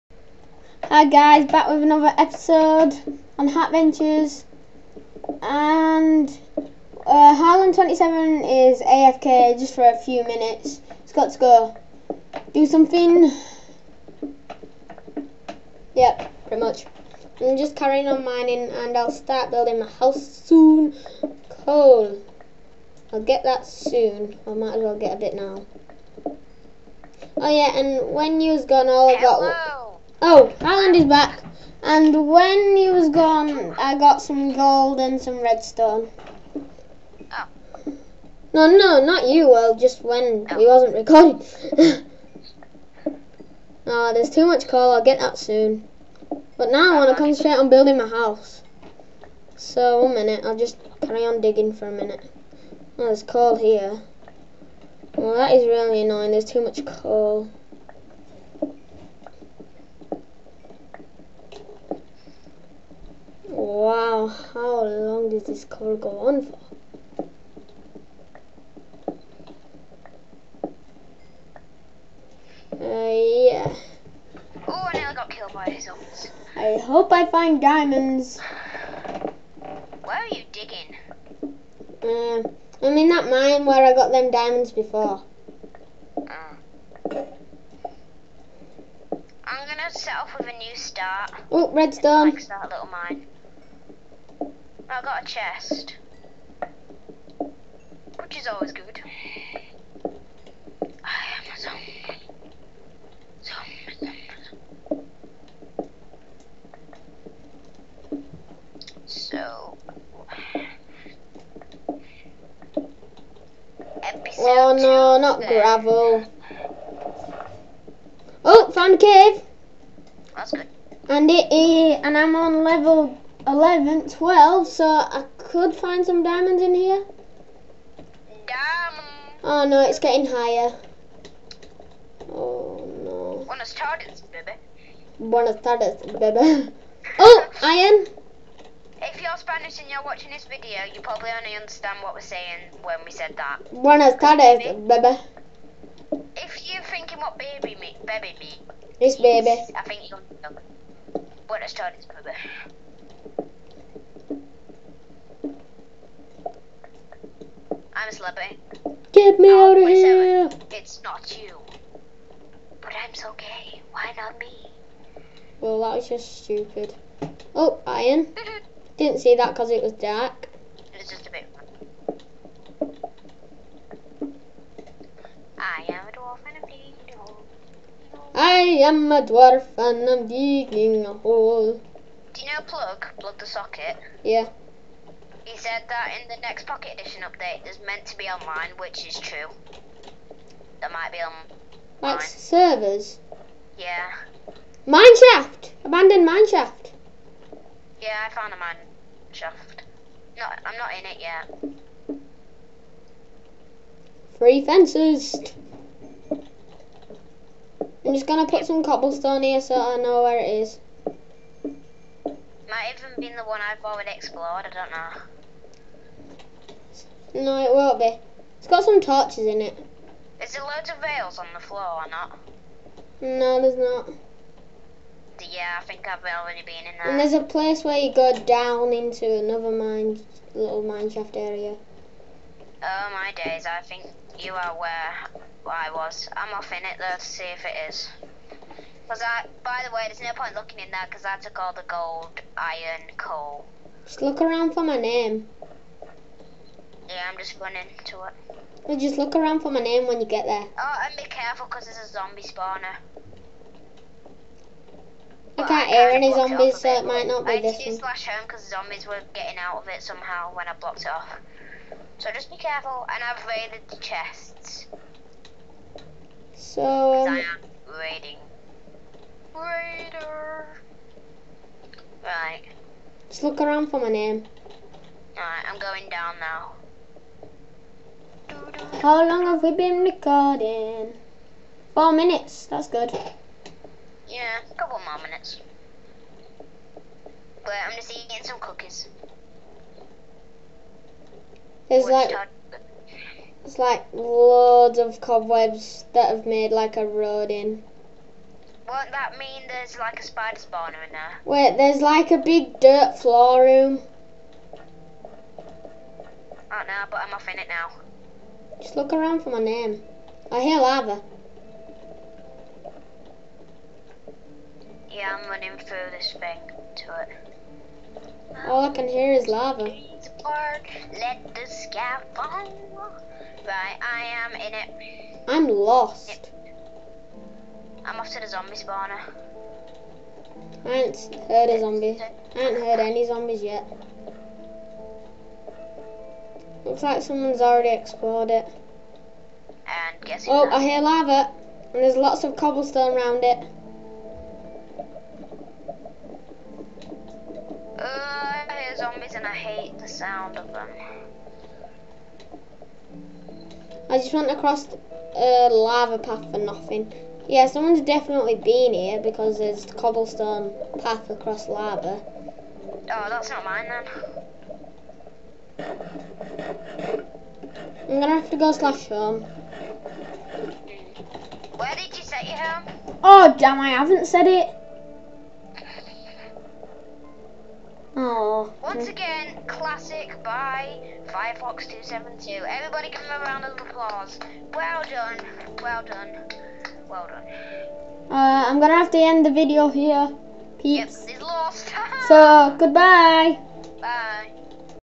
me and friend play hatventures part 2: Searching a mineshaft
sos i will not put the ip in the description because the owners do not aprove it